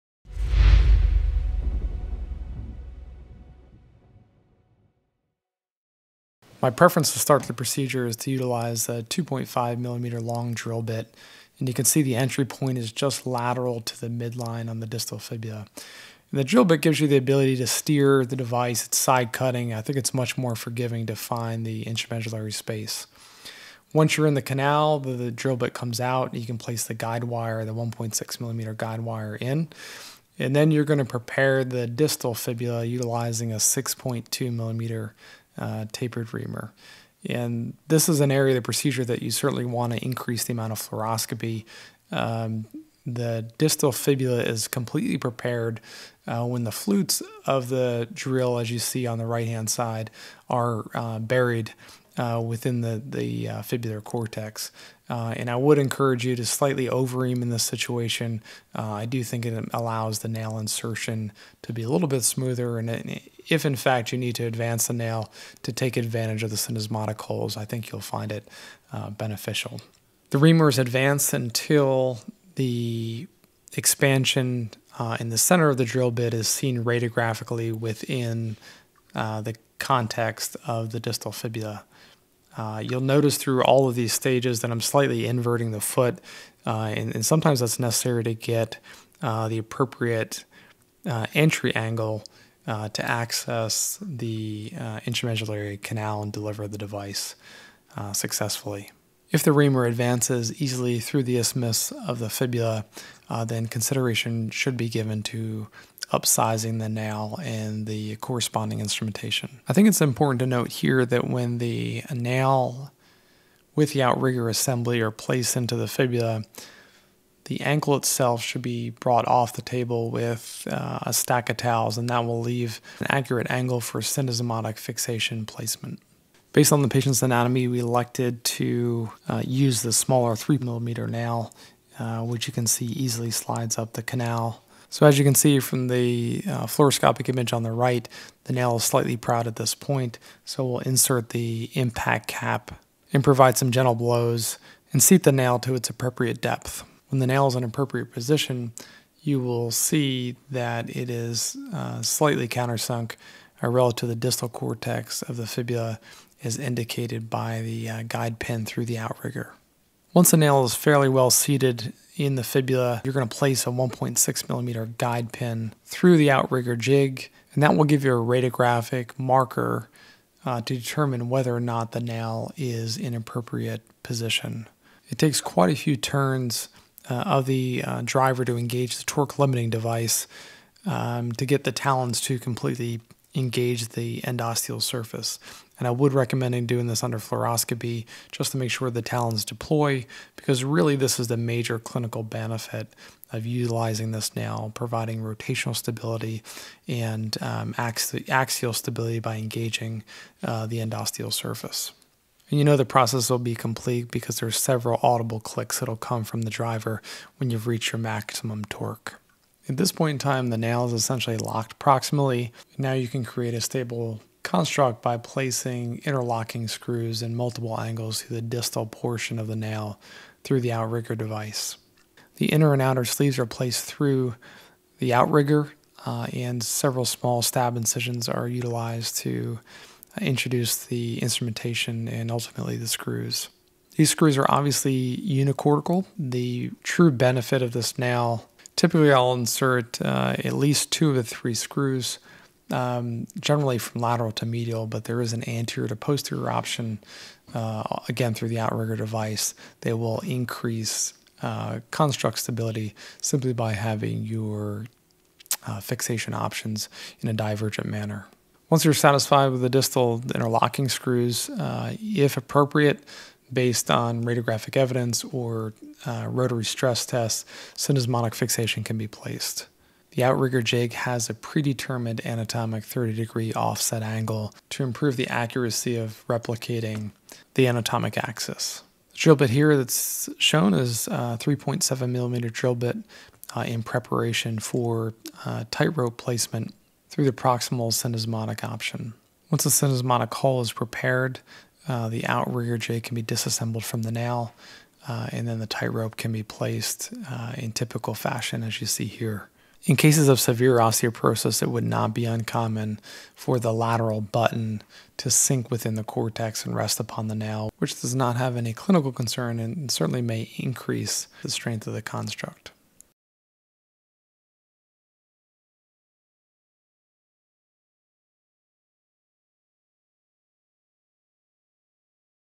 FibuLock® Nail With the TightRope® Fixation System
Justin Fleming, DPM, (Somerset, NJ) demonstrates the FibuLock® fibular nail and Knotless TightRope® fixation for fibular fractures. Dr. Fleming reviews tips and pearls including entry point, nail positioning, and syndesmotic TightRope fixation.